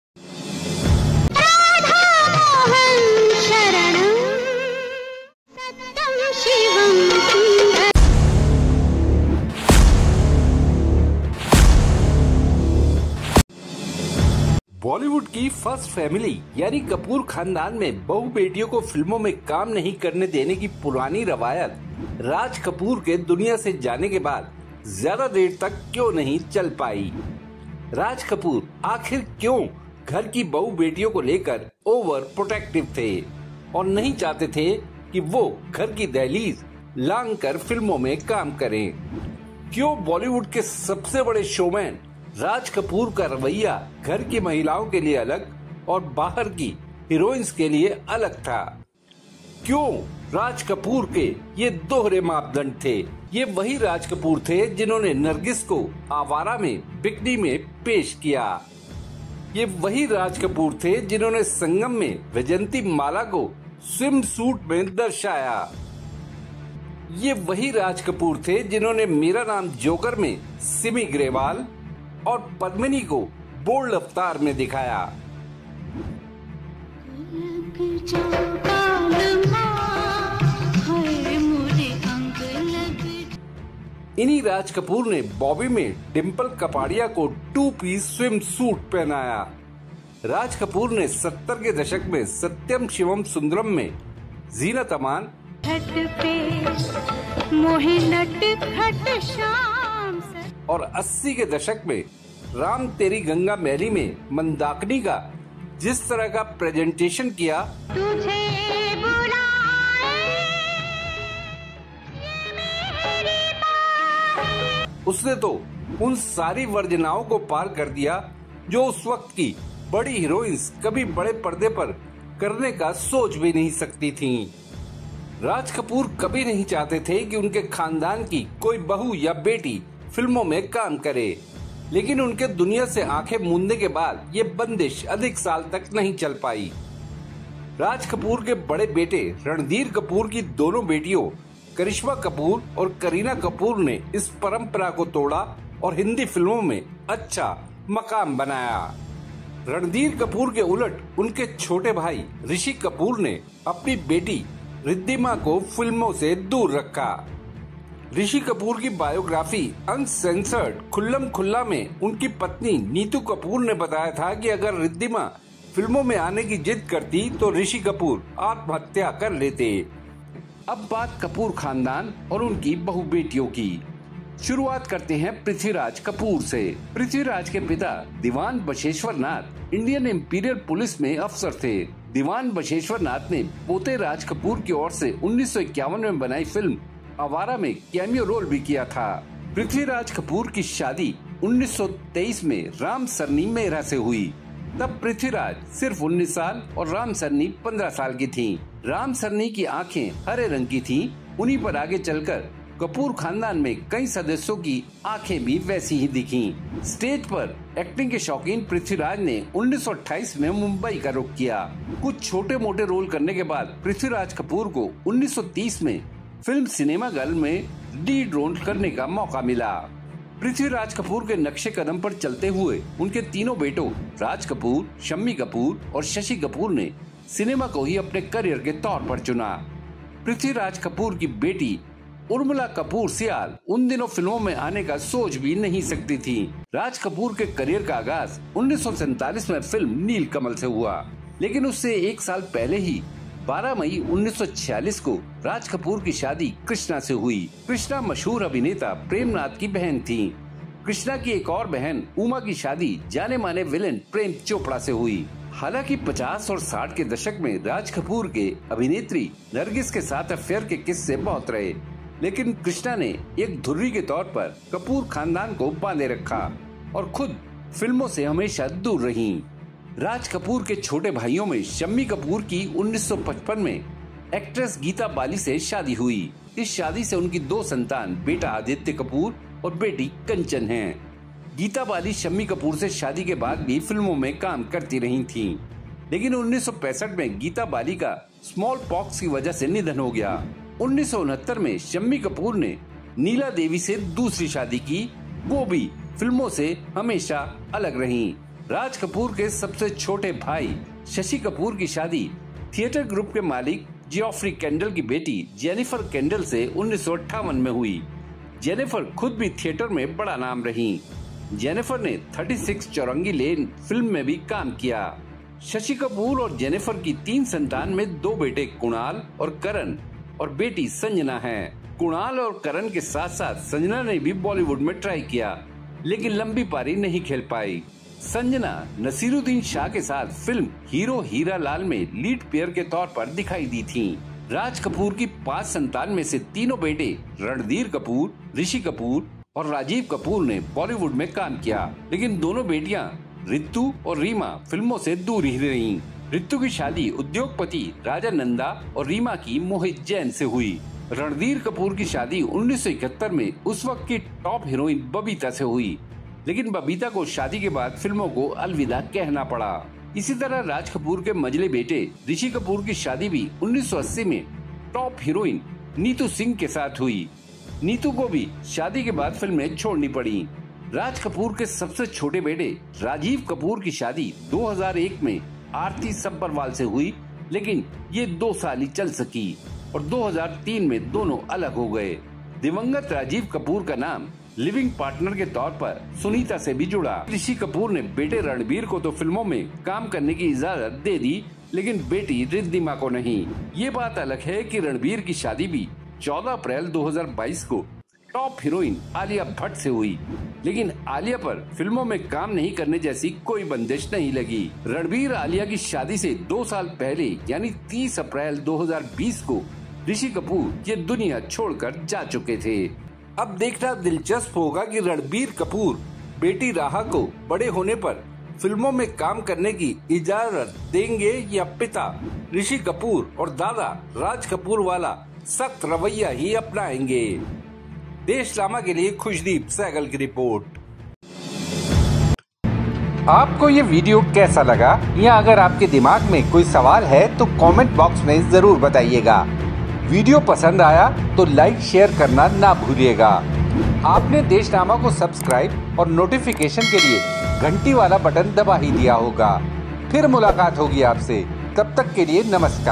Raj Kapoor 100 | Birth Centenary | Double Standards | Family Ladies & Heroines | Biography in Hindi
Raj Kapoor 100 | Birth Centenary | Double  Standards | Family Ladies & Heroines | Biography in Hindi - Deshnama Hindi News

Why did the old tradition of not allowing own ladies to work in films in Bollywood's Kapoor family, not last long after Raj Kapoor's death? Why was Raj Kapoor over-protective of his daughters-in-law and daughters and did not want them to step out of the house and work in films? Why Raj Kapoor's attitude was different for the women of the house and for the heroines outside. This was the same Raj Kapoor who presented Nargis in bikini in ‘Awara’ way back in 1951. Vaijayanti Mala was shown in a swimsuit in Sangam. Simi Grewal and Padmini were shown in bold avatars in Mera Naam Joker and Dimple Kapadia was shown wearing a two-piece swimsuit in Bobby. The way Raj Kapoor presented Zeenat Aman in Satyam Shivam Sundar in the 70s and Mandakini in Ram Teri Ganga Maili in the 80s crossed all the taboos that the big heroines of that time could not even think of doing on the big screen. In this story of Deshnama, we talk about Raj Kapoor's double standard personality with a look at the 94-year history of the Kapoor family.

बॉलीवुड की फर्स्ट फैमिली यानि कपूर ख़ानदान में बहू बेटियों को फिल्मों में काम नहीं करने देने की पुरानी रवायत राज कपूर के दुनिया से जाने के बाद ज़्यादा देर क्यों नहीं चल पाई. राज कपूर आख़िर क्यों घर की बहू बेटियों को लेकर ओवर-प्रोटेक्टिव थे और नहीं चाहते थे कि वो घर की दहलीज़ लांघ कर फिल्मों में काम करें. क्यों राज कपूर का रवैया घर की महिलाओं के लिए अलग और बाहर की हीरोइन्स के लिए अलग था. ये वही राज कपूर थे जिन्होंने नर्गिस को आवारा में बिकनी में बड़े पर्दे पर पेश किया. संगम में वैजयंती माला को स्विम सूट में दर्शाया. मेरा नाम जोकर में सिम्मी ग्रेवाल और पद्मिनी को बोल्ड अवतार में दिखाया तो बॉबी में डिंपल कपाड़िया को टूपीस स्विम सूट पहनाया. राज कपूर ने सत्तर के दशक में सत्यम शिवम सुंदर में जीनत अमान और अस्सी के दशक में राम तेरी गंगा मैली में मंदाकिनी का जिस तरह का प्रेजेंटेशन किया, उसने तो उन सारी वर्जनाओं को पार कर दिया जो उस वक्त की बड़ी हीरोइन्स कभी बड़े पर्दे पर करने का सोच भी नहीं सकती थी. कपूर खानदान के 94 साल के इतिहास पर एक नज़र के साथ राज कपूर की डबल स्टैंडर्ड पर्सनेल्टी पर बात देशनामा की इस स्टोरी में.